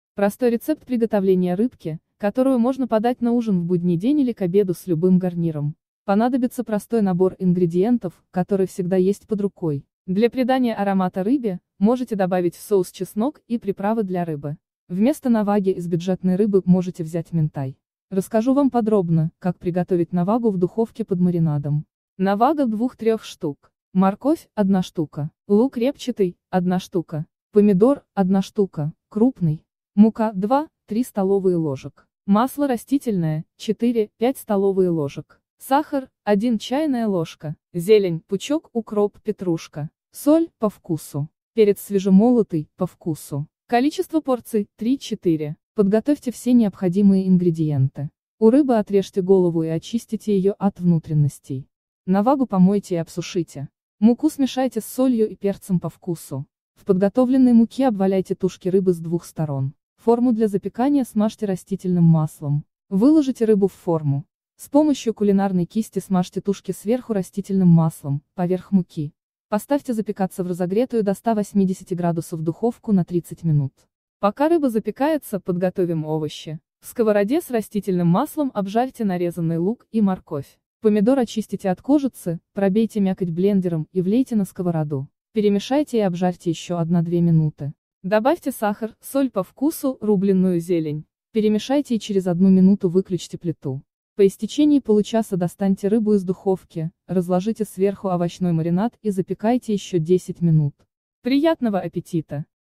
Навага в духовке под маринадом
Простой рецепт приготовления рыбки, которую можно подать на ужин в будний день или к обеду с любым гарниром. Понадобится простой набор ингредиентов, который всегда есть под рукой.